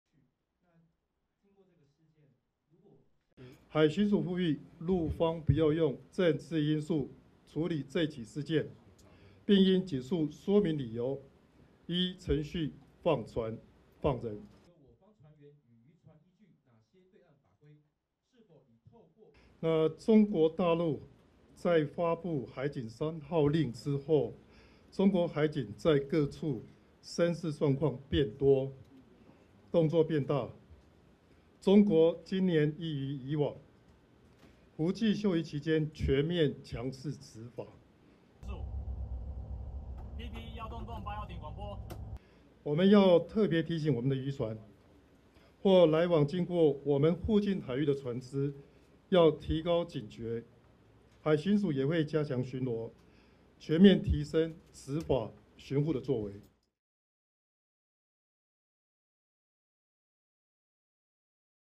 台湾呼吁中国立即释放被中国海警扣押的台湾渔船及船员
台湾7月3日敦促中国立即释放被中国海岸警卫队扣押的一艘台湾渔船及其船员。台湾海巡署副署长谢庆钦在记者会上呼吁中方不要用“政治因素处理这起事件”，并尽快说明理由，放船、放人。他还表示中国海警今年在伏季休渔期间全面强势执法，台湾渔船应提高警惕。报道：